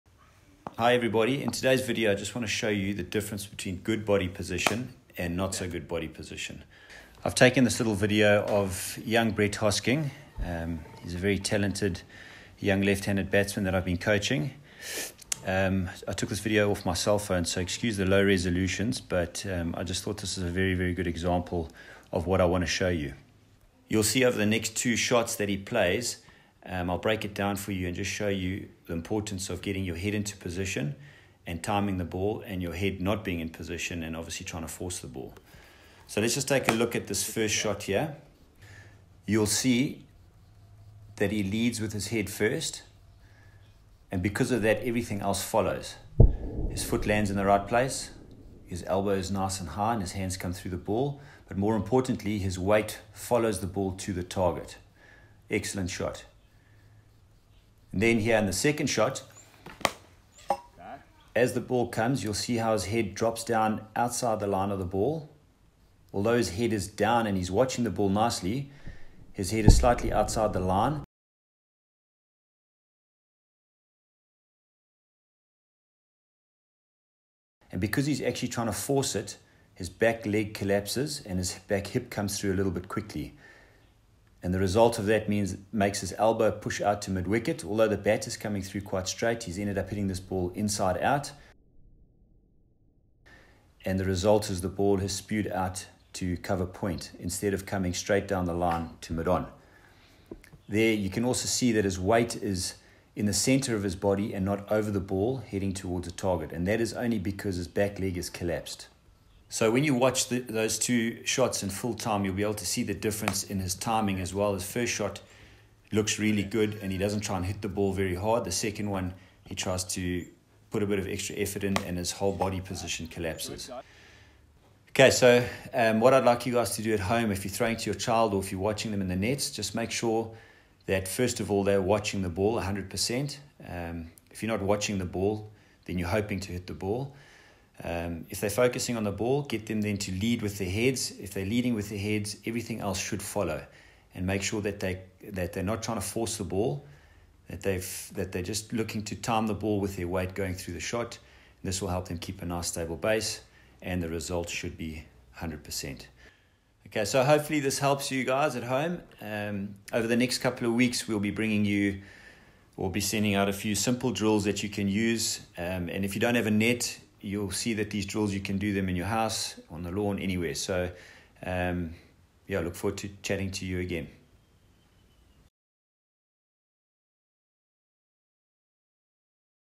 🏏 Cricket Batting: How to Recognize Good vs Bad Body Position:
When you're having a cricket batting net with your son or daughter, do you know when they have a good or bad body position?

Do you know what the difference is?

This is just a quick video example to show you the difference between a good position and then a bad position, and why each is right and wrong.